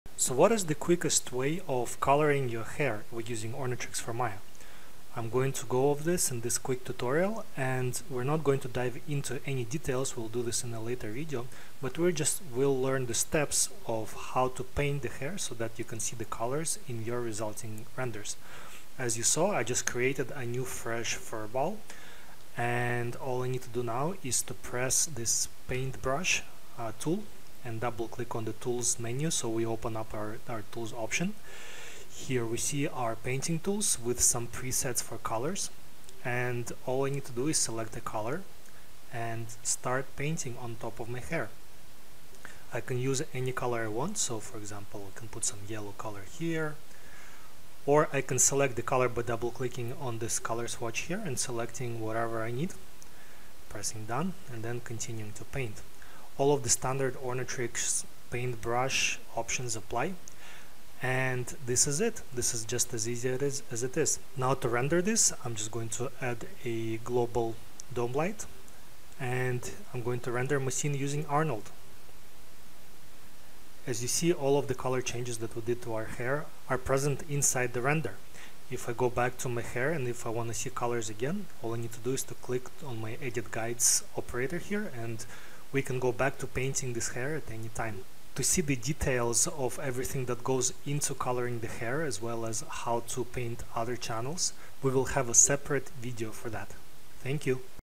Ornatrix Maya: Painting the hair with color
A quick tutorial about coloring the hair using a paint brush with Ornatrix Maya.

Ornatrix is a complete solution for creating hair, fur, and feathers in Autodesk Maya.